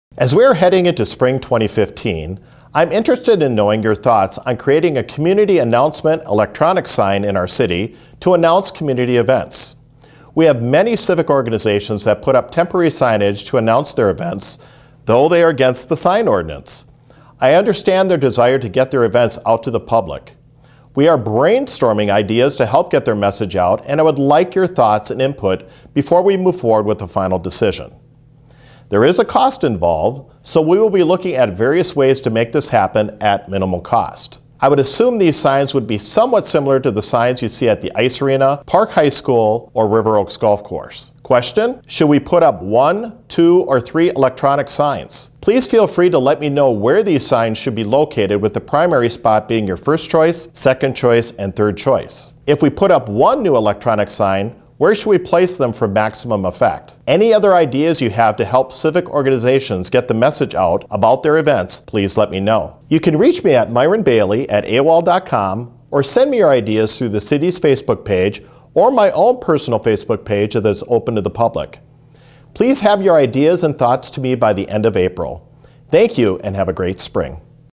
Cottage Grove Mayor's Moment March 2015
Mayor Myron Bailey is seeking input from Cottage Grove residents about a new way to communicate information to the community.